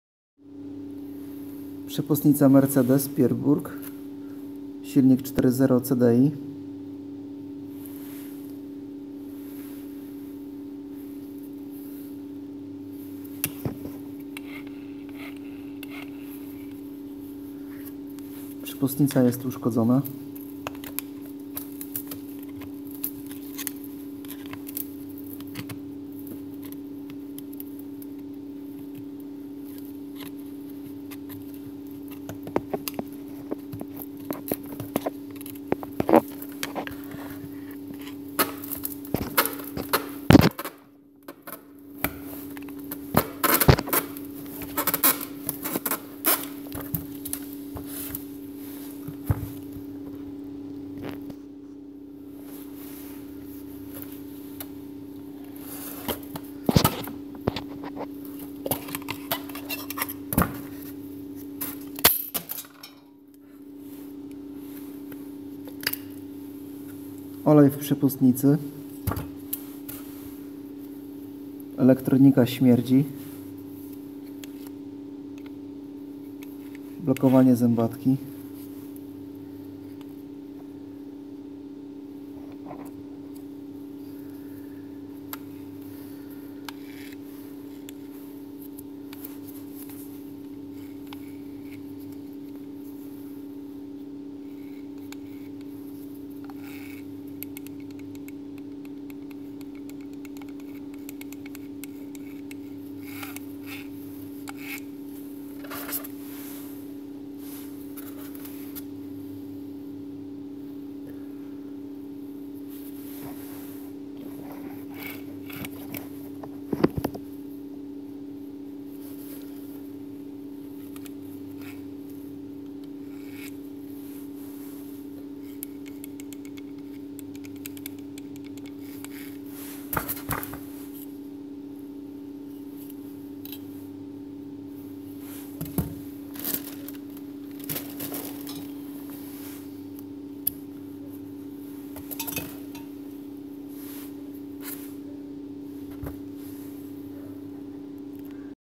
Uszkodzona przepustnica powietrza mercedes ML GL 4.0 CDI 4.2 drosselklape defekt
Przepustnica mercedes 4.0cdi uszkodzona elektronika. Olej w środku spowodował uszkodzenia elektroniki oraz wyrobienie przekładni.